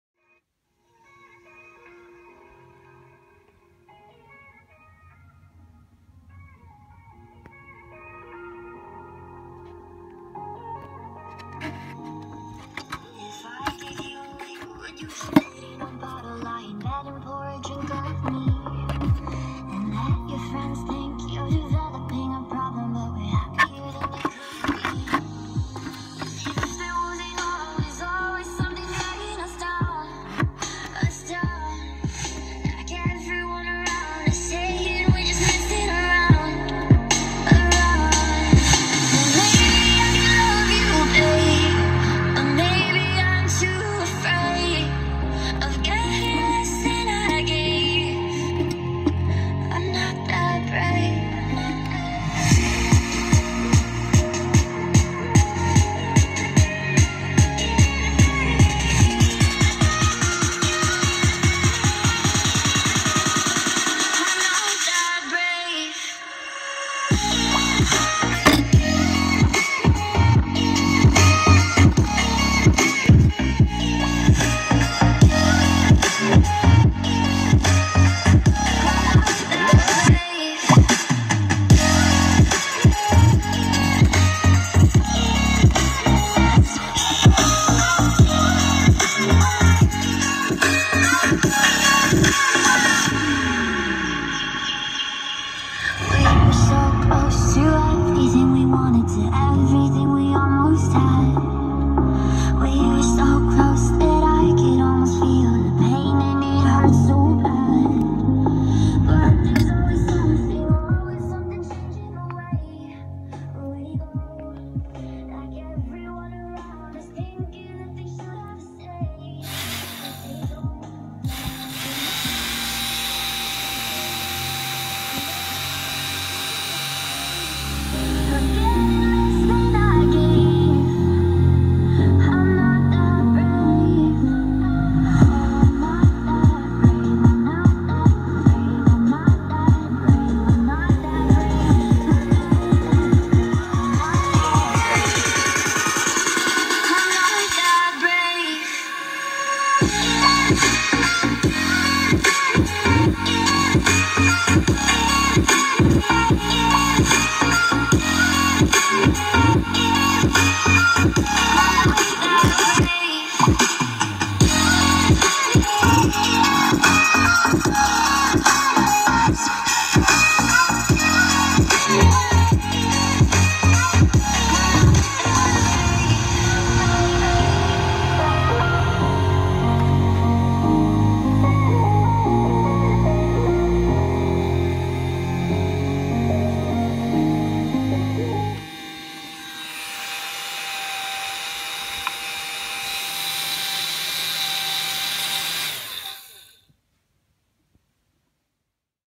Amplifier TDA7297 Sound Test + Heat-sink Fan Cooling System
Hello everyone, and welcome back to another video.
The speaker used in this video is 50W, running on a 30W amplifier.